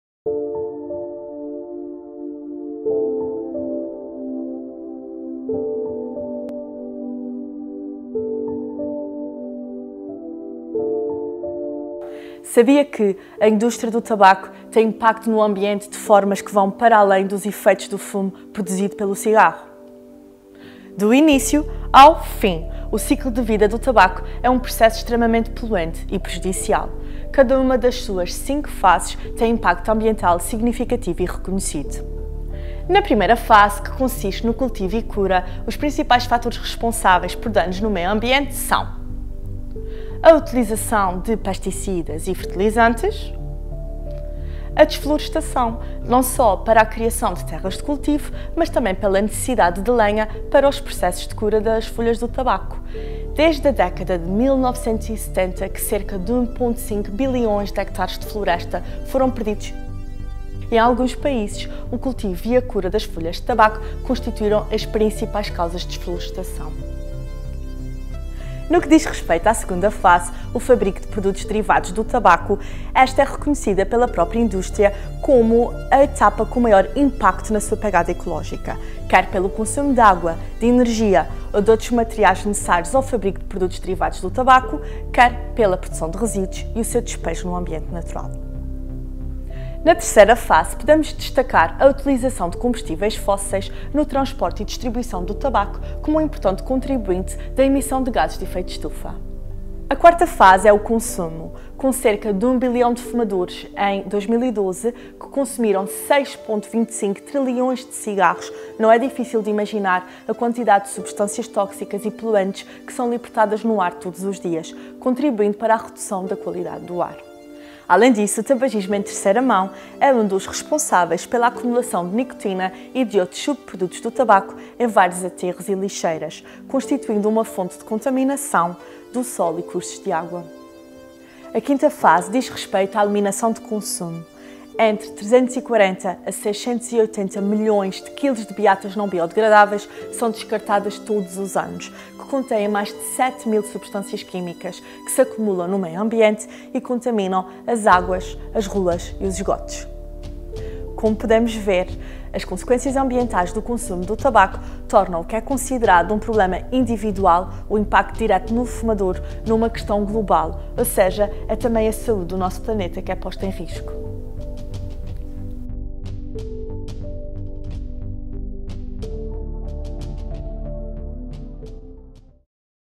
IMPACTO AMBIENTAL DO TABAGISMO - Dra. Cláudia Matos
A Comissão de Trabalho de Tabagismo da Sociedade Portuguesa de Pneumologia acaba de lançar mais um vídeo de dicas e motivação para deixar de fumar. Desta vez, a protagonista é a Dr.ª Cláudia Matos que fala do impacto ambiental do tabaco. Assista e partilhe.